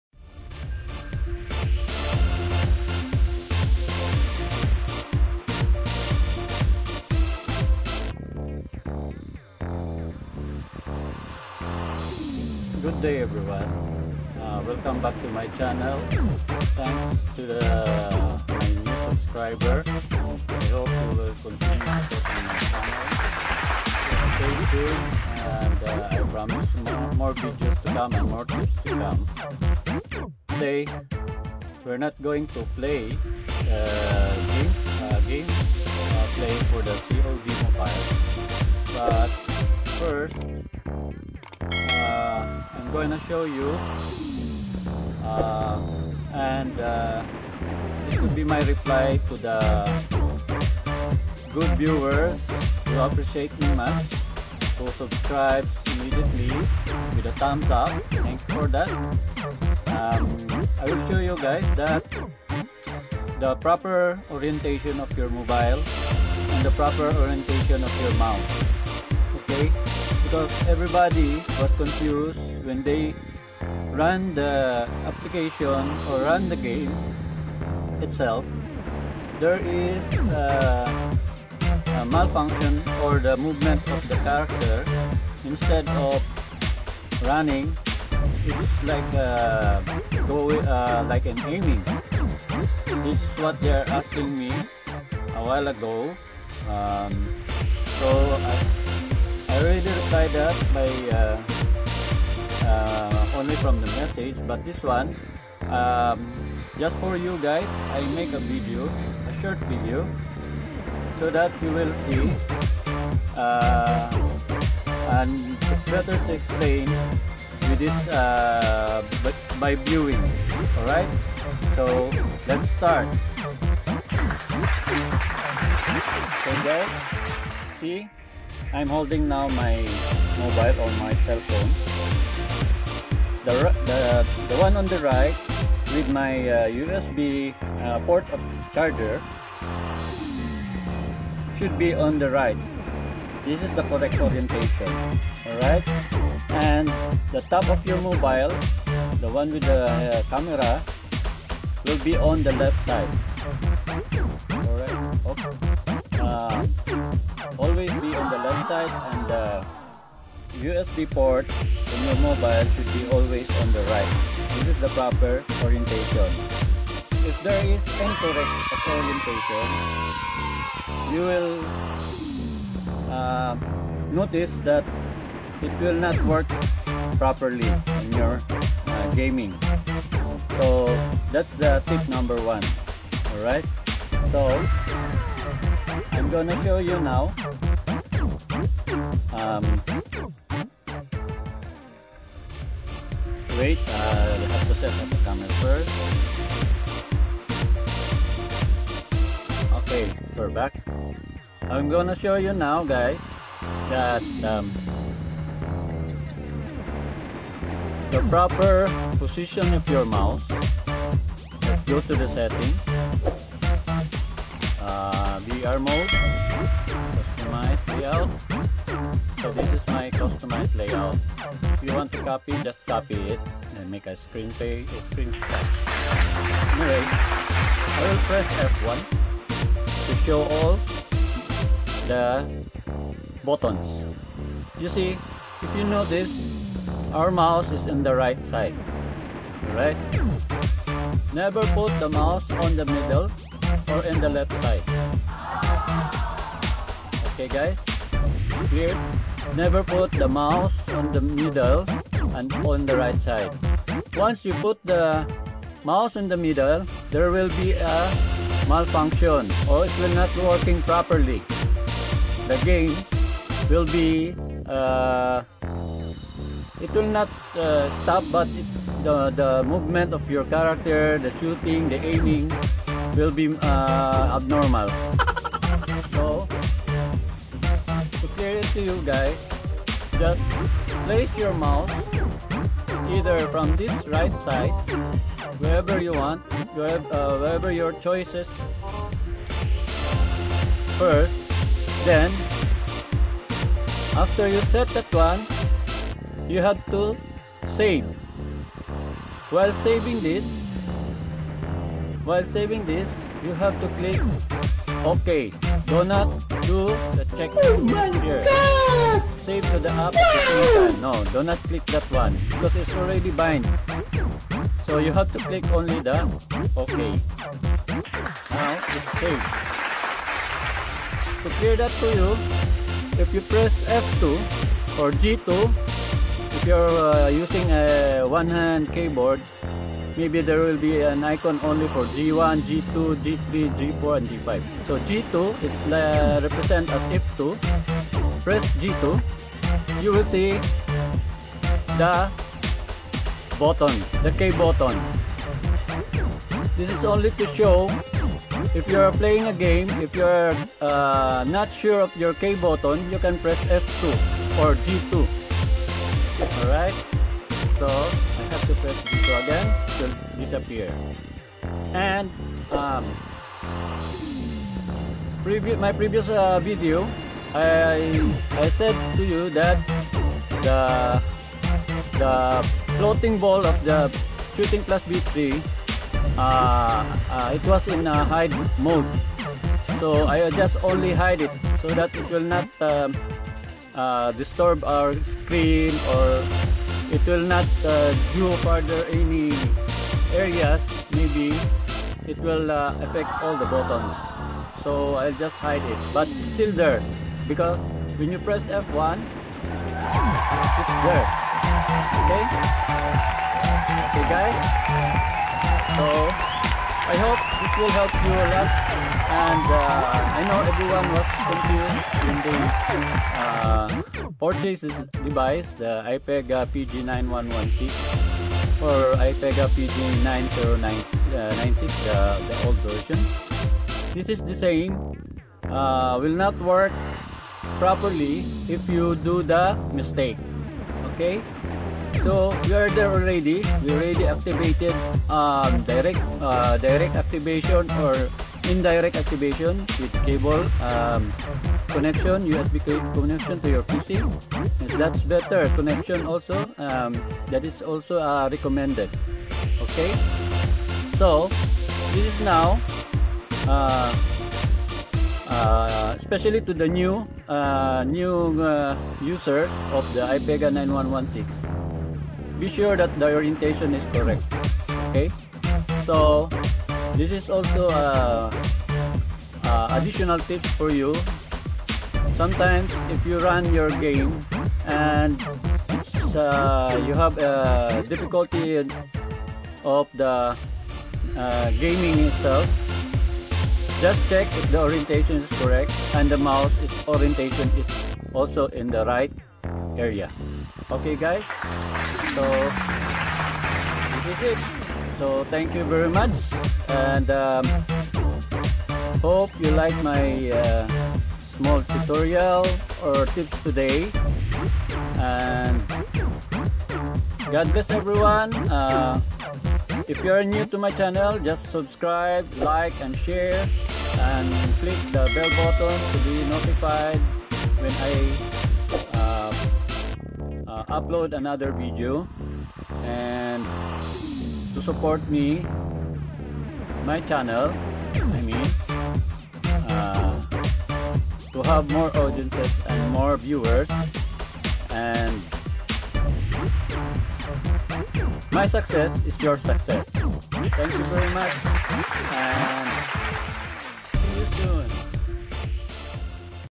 Ipega 9116 and ShootingplusV3 Secret' Guide! problem solved!
Tips & Guide for Users of Ipega 9116 keyboard & mouse converter and ShootingplusV3 apps  facing problems opening the game not working properly after complete setup.